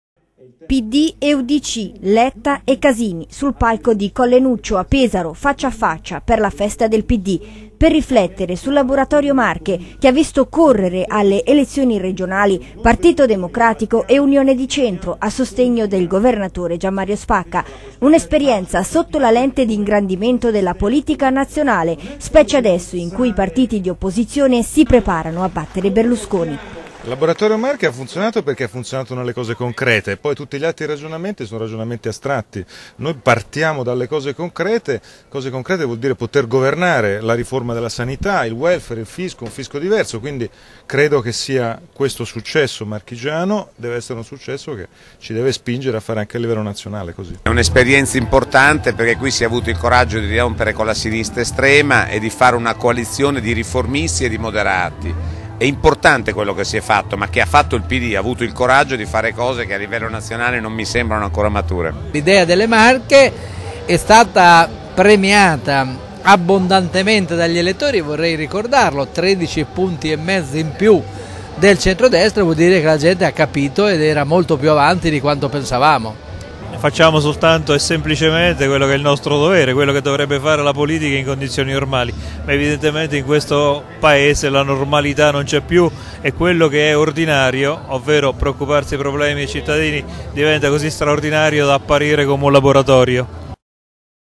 IL LABORATORIO MARCHE
Sul palco di Pesaro, per la festa del Pd, Letta e Casini ragionano sul "Laboratorio Marche", allenza, quella che ha portato Spacca alla vittoria alle regionali, da replicare in vista delle elezioni nazionali?
Il servizio di Linda Cittadini.